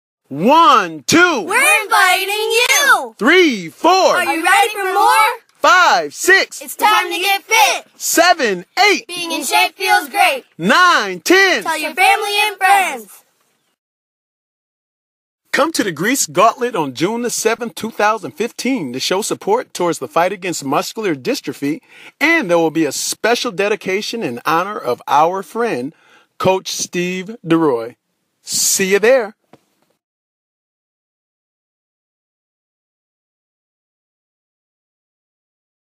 New Recruit Sports LLC Presents: 2ND ANNUAL GREECE GAUNTLET 5K OBSTACLE COURSE MUD RUN
Run, crawl, sprint and splash through the Gauntlet, Greece's first ever family friendly out obstacle course, The Gauntlet is designed for atheletes of all levels. Over 16 obstacles are incorporated into this 5 K mud run. This is a great opportunity to challenge yourself or continue your training. You're gonna get wet, muddy and tired, all while having the time of your life.
There will also be a dedication giving an honor of Coach Steve Deroy.